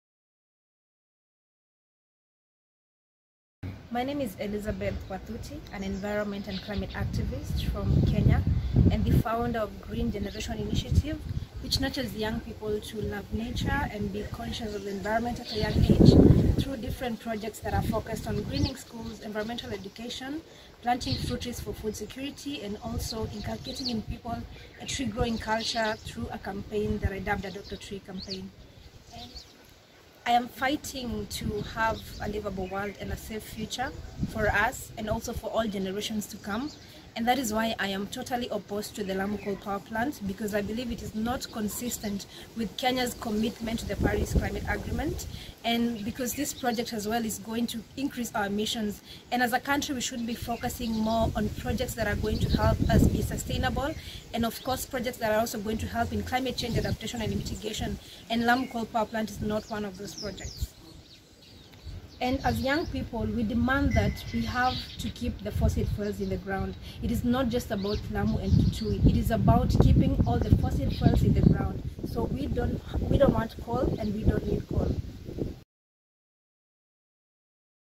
My Position on The Lamu Coal Power Plant - Interview with deCOALonize Kenya.
We must keep the fossil fuels in the ground.